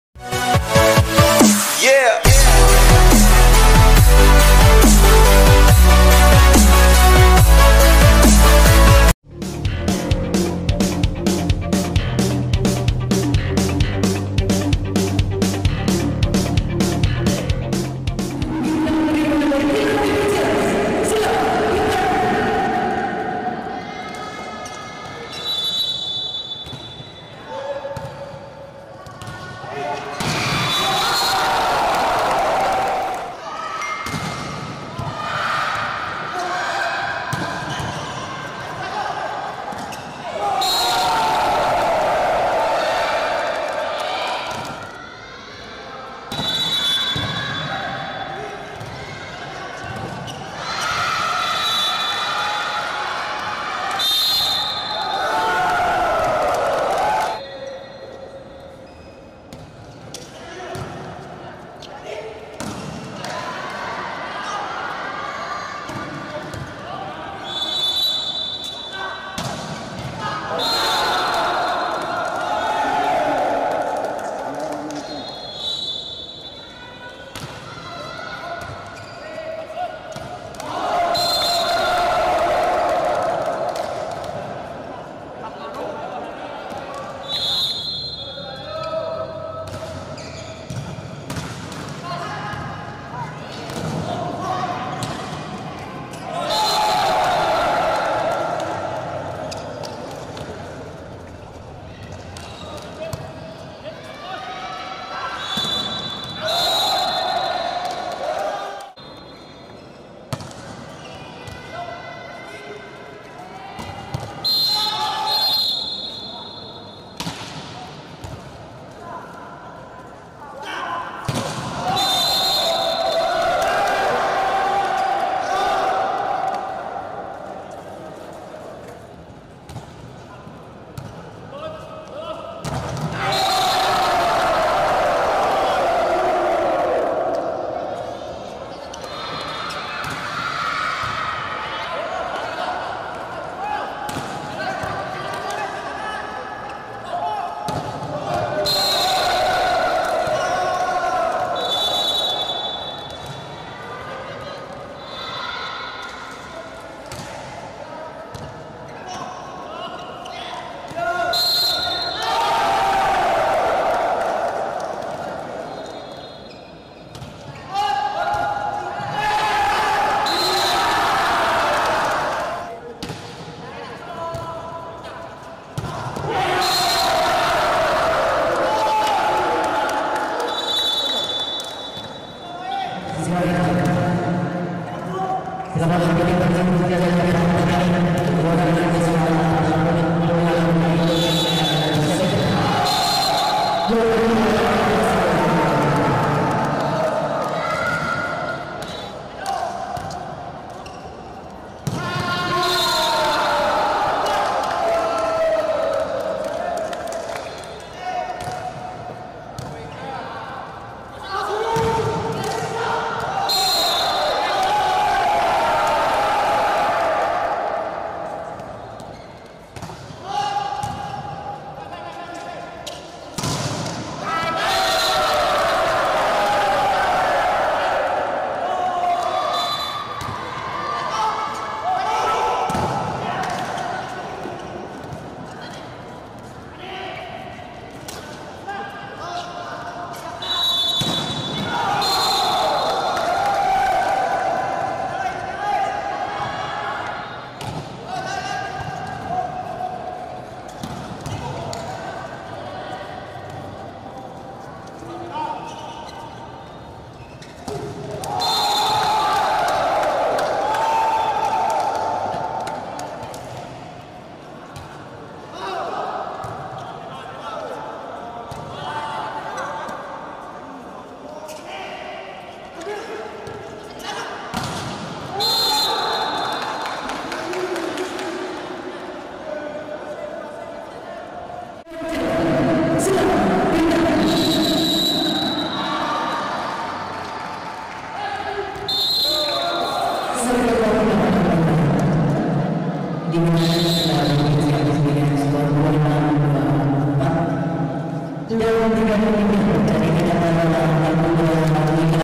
highlights | Kejurnas BolaVoli Indor Antar Club U-17 Sidoarjo | Mitra Prima Banten vs Kila Vb Ciamis
Kejuaraan nasional bola voli indor antar club U-17 Sidoarjo,diadakan oleh PBVSI